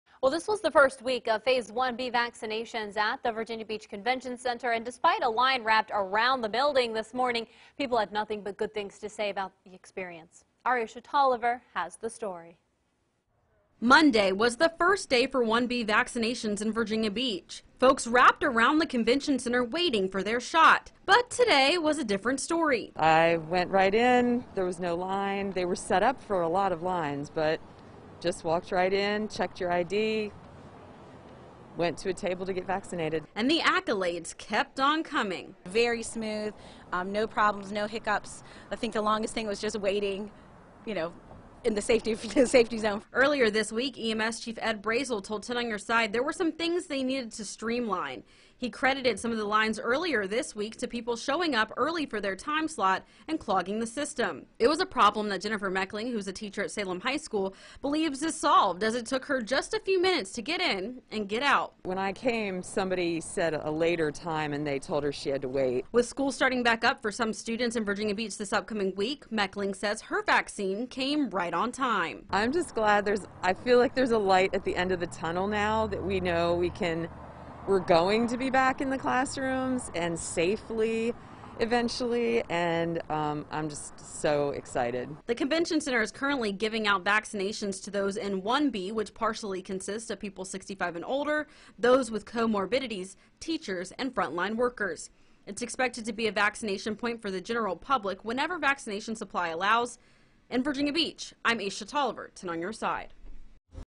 Virginia Beach starts COVID-19 vaccinations for phase 1b recipients
WAVY News 10's Aesia Toliver reports.